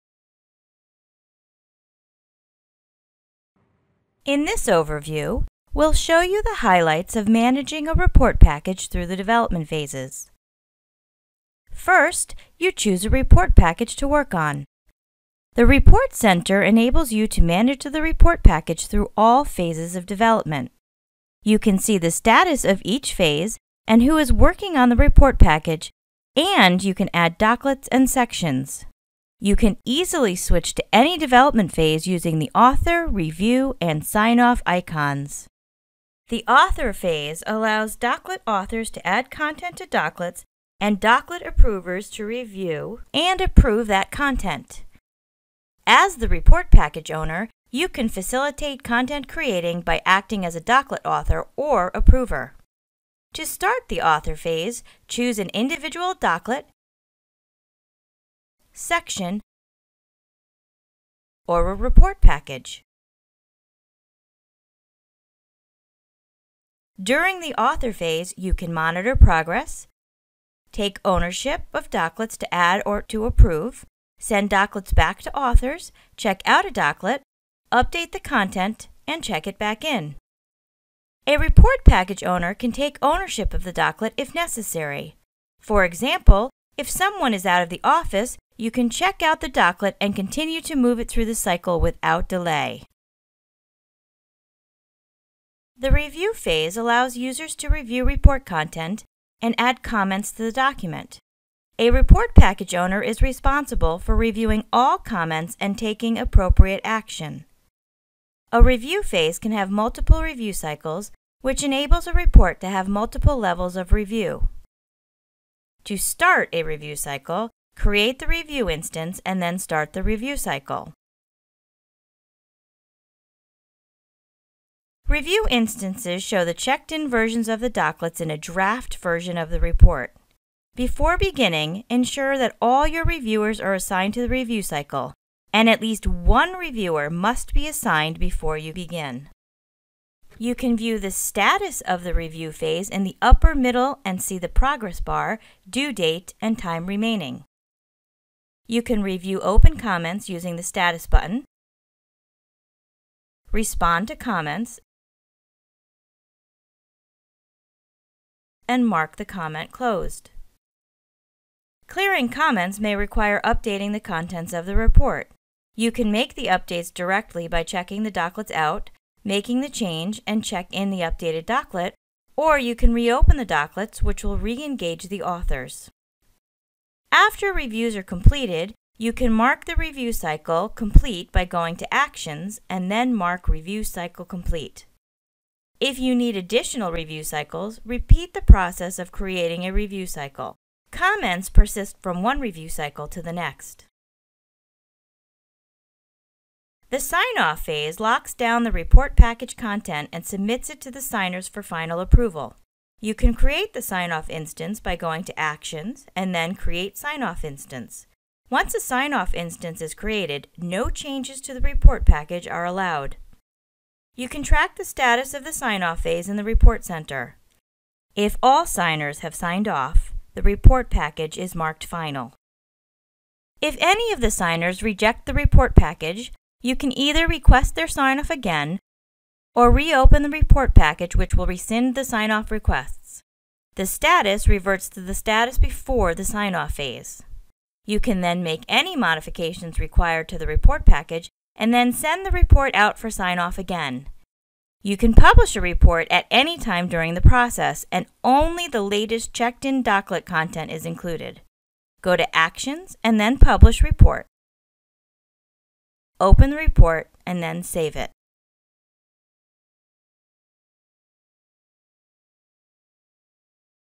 Managing Report Packages
In this video, report package owners learn how to manage the Report Package through its development phases.